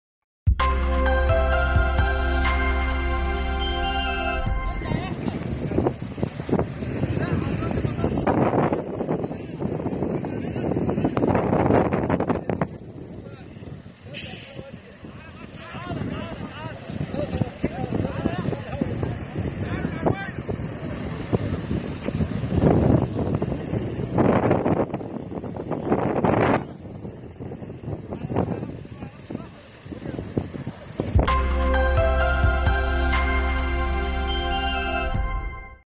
لحظة اندلاع الحريق في حقل بترول "أم عسل" بمدينة رأس سدر
تقرير: أية السيد
مونتاج : أحمد عبدالشفيع